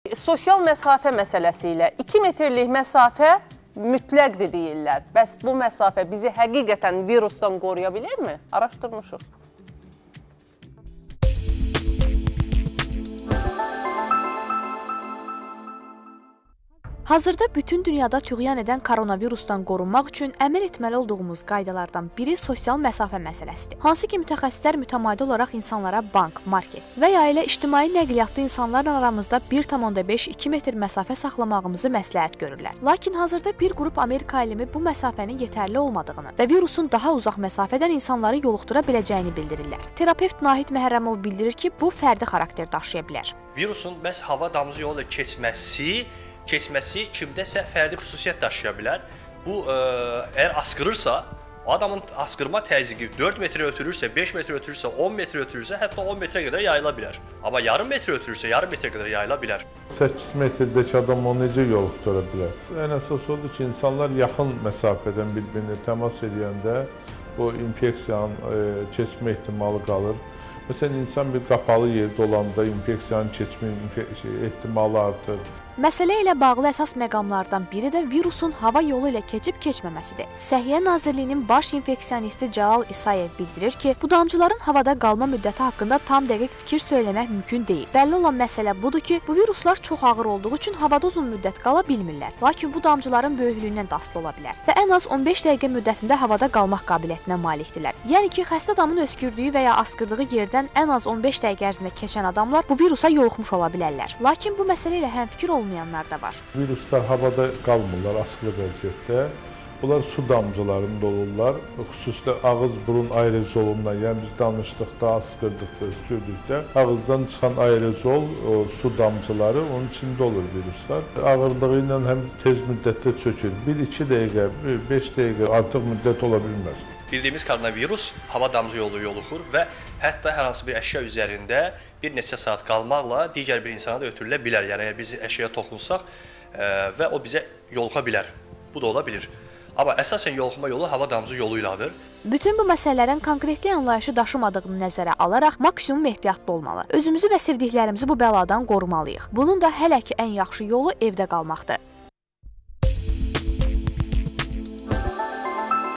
Iki metrlik ara mesafesi bizi qoruyurmu? | Gune Dogru
Hazırda bütün dünyada tüğyan edən virusdan qorunmaq üçün əməl etməli olduğumuz məsələrdən biri sosial məsafə məsələsidir. Hansı ki, mütəxəssislər mütamadi olaraq insanlara bank, market və ya elə ictimai nəqliyyatda insanlarla aramızda 1,5- 2 metr məsafə saxlamağımızı məsləhət görürlər. Lakin hazırda bir qrup Amerika alimi bu məsafənin yetərli olmadığını və virusun daha uzaq məsafədən insanları yoluxdura biləcəyini bildirirlər. Terapevt Nahid Məhərəmmov bildirir ki, bu fərdi xaraketer daşıya bilər. Məsələ ilə bağlı əsas məqamlardan biri də virusun hava yolu ilə keçib keçməməsidir. Səhiyyə Nazirliyinin baş infeksionisti Cəlal İsayev bildirir ki: Bu damcıların hava da qalma müddəti haqqında tam dəqiq fikir söyləmək mümkün deyil. Bəlli olan məsələ budur ki, bu viruslar çox ağır olduğu üçün hava da uzun müddət qala bilmirlər. Lakin bu damcıların böyüklüyündən də asıla bilər və ən az 15 dəqiqə müddətində hava da qalmaq qabiliyyətinə malikdirlər. Yəni ki, xəstə adamın öskürdüyü və ya asqırdığı yerdən ən az 15 dəqiqə ərzində keçən adamlar bu virusa yoluxmuş ola bilərlər. Lakin bu məsələ ilə həmfikir olmayanlarda var. Bütün bu məsələlərin konkretlik anlayışı daşımadığını nəzərə alaraq maksimum ehtiyatlı olmalı özümüzü və sevdiklərimizi bu bəladan qorumalıyıq. Bunun da hələki ən yaxşı yolu evdə qalmaqdır